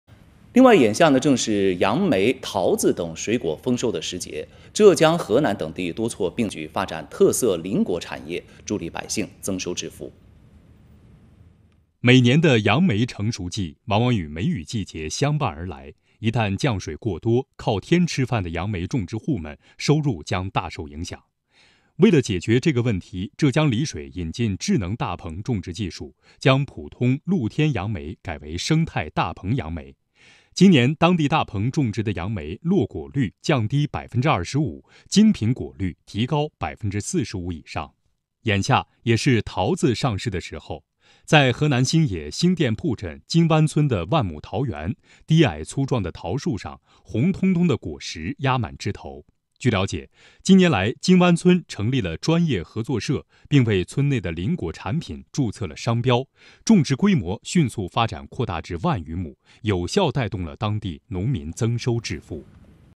[正点财经]“三夏”农忙正当时 浙江丽水：智能大棚助力杨梅产业发展|CCTV财经
“三夏”农忙正当时·浙江丽水：智能大棚助力杨梅产业发展。